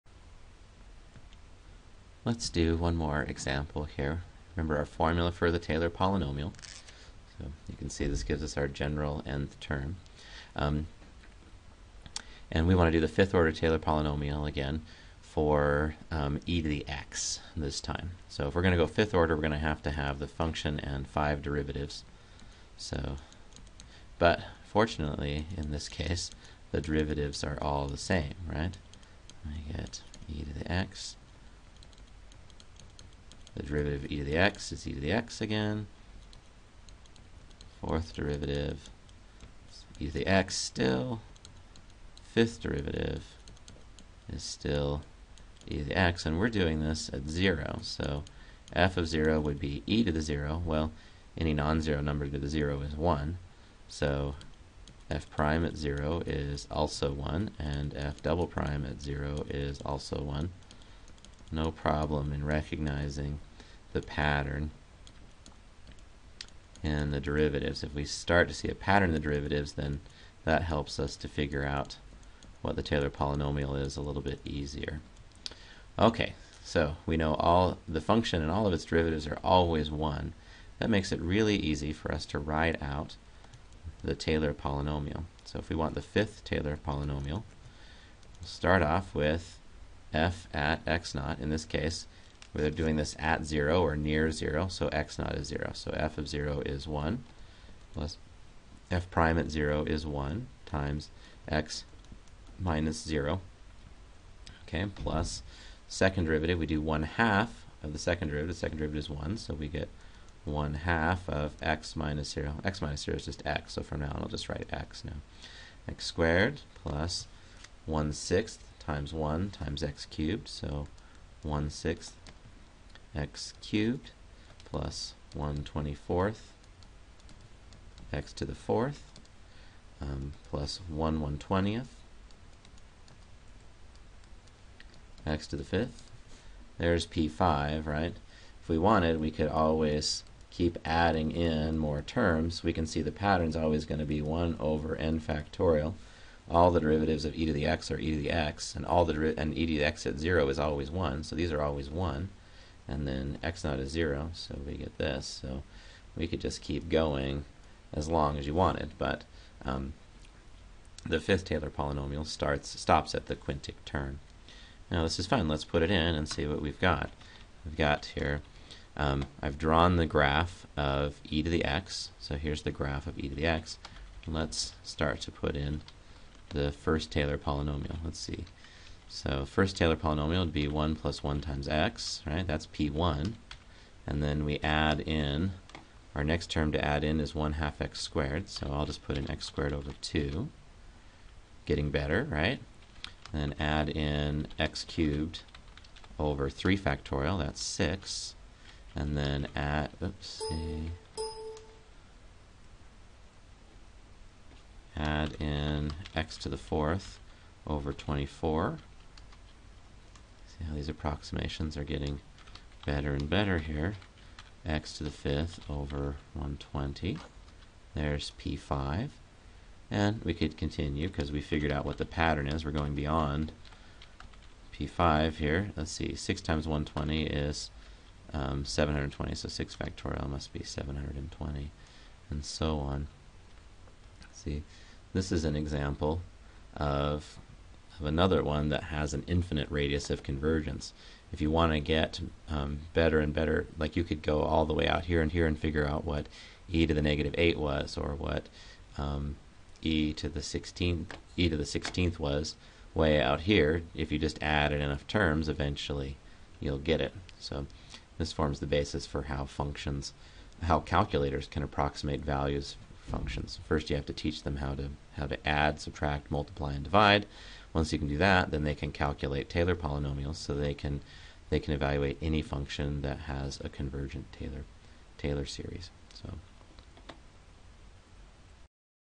Taylor Polynomial for Exp
Finding the 5th order Taylor Polynomial for Exp